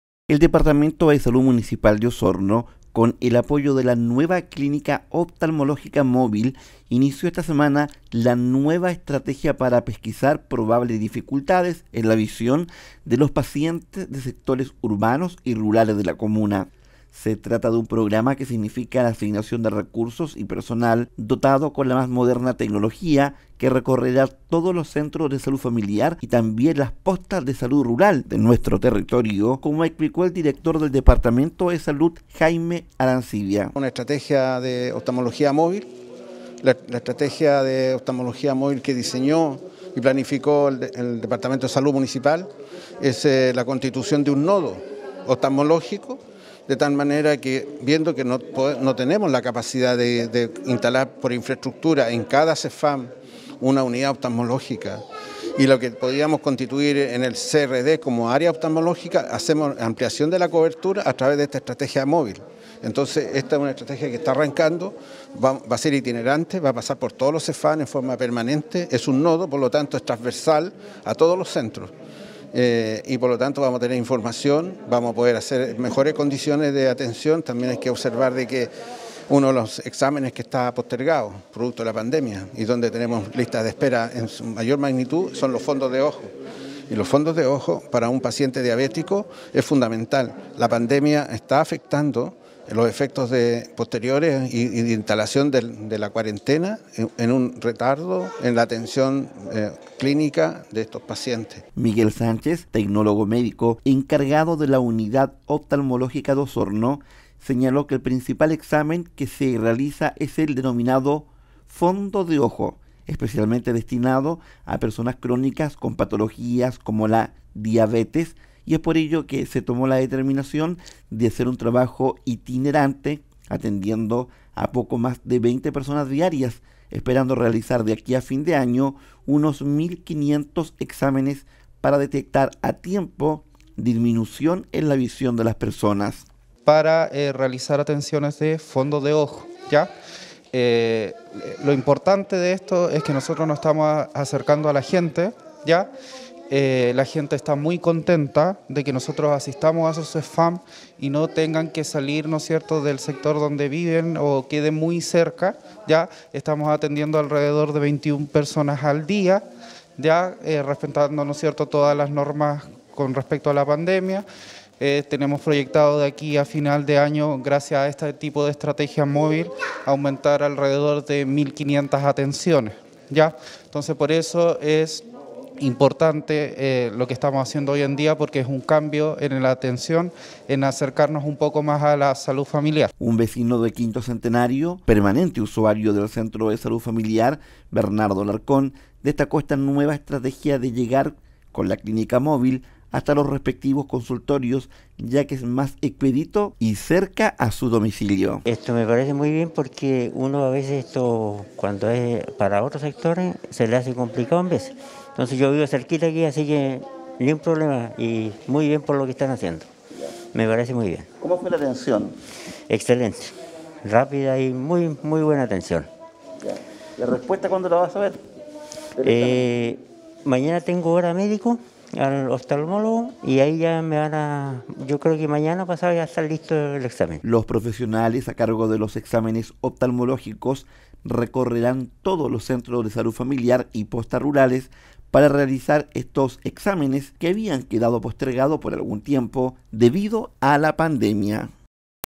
Ofrecerán atención oftalmológica móvil en la comuna de Osorno
Se trata de un programa que significa la asignación de recursos y personal, dotados con la más moderna tecnología que recorrerá todos los centros de salud familiar y también las postas de salud rural de nuestro territorio, explicó el director de la salud Jaime Arancibia.